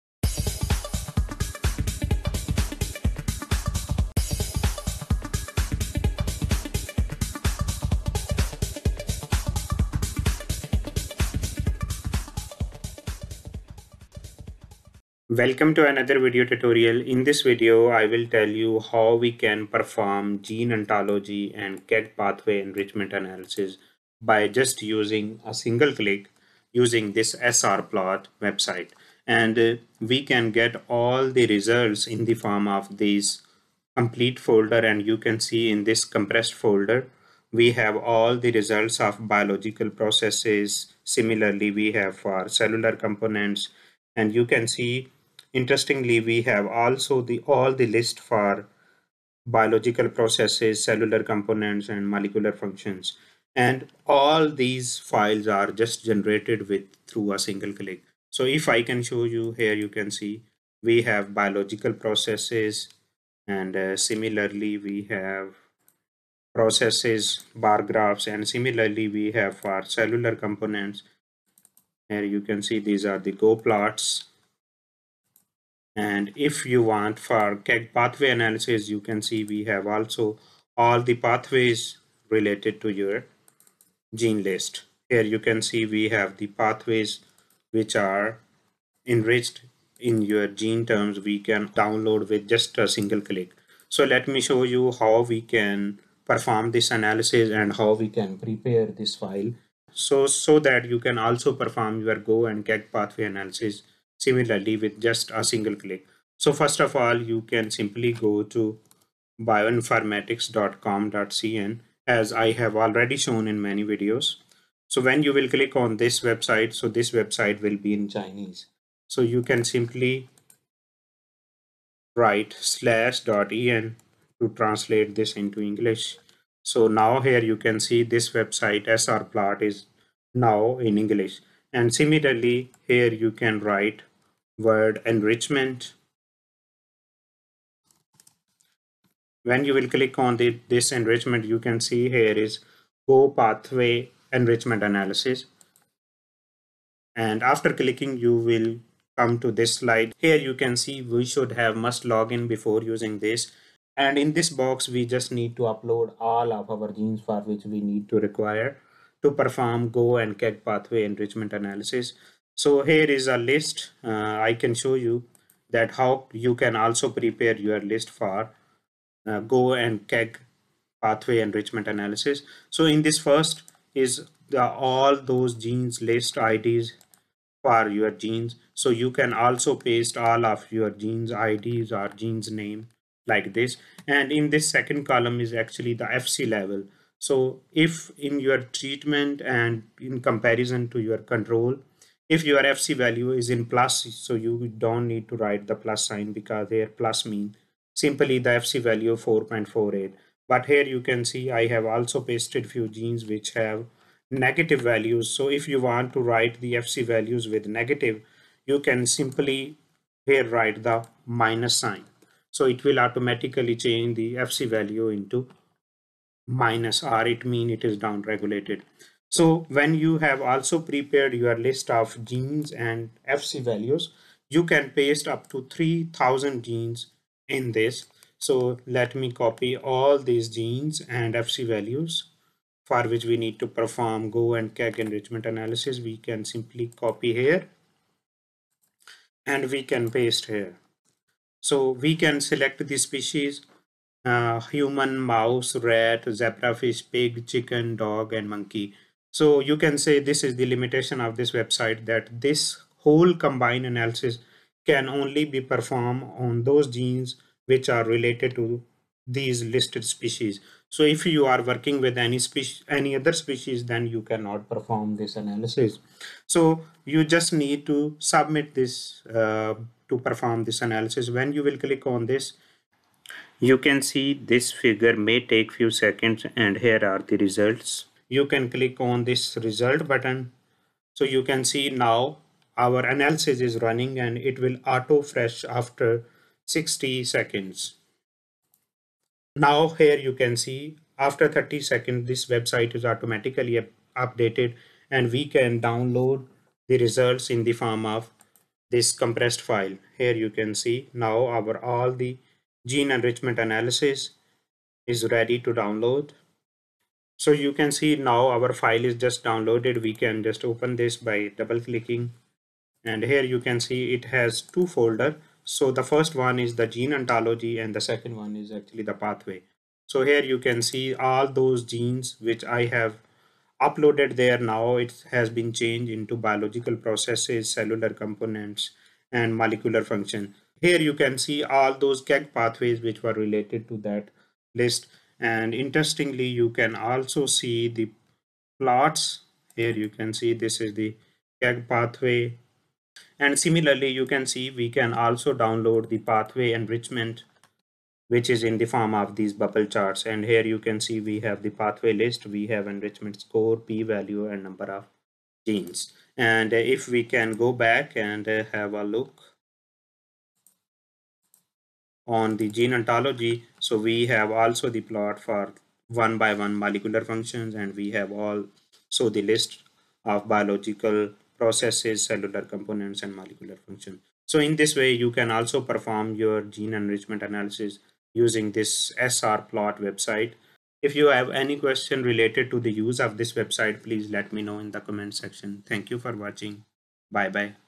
How to perform gene enrichment (GO and KEGG pathways) analysis with SR plot
In this video, I have performed gene enrichment analysis gene ontology, and KEGG pathway using SR online web tool.

🔴If you are a graduate student and trying to learn something new in easy way, Watch my playlists